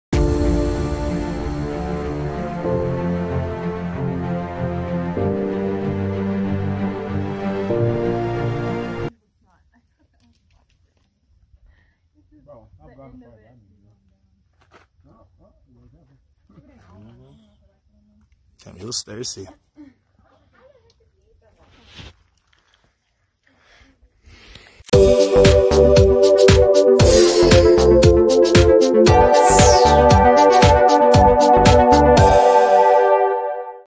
Thirsty squirrel gratefully drinks water from hikers In Grand Canyon in Arizona
While hiking at the Grand Canyon in Arizona, John came across a thirsty squirrel on July 1. John offers the squirrel some water, and the squirrel gratefully accepts a few sips as it grips the plastic bottle with its small hands.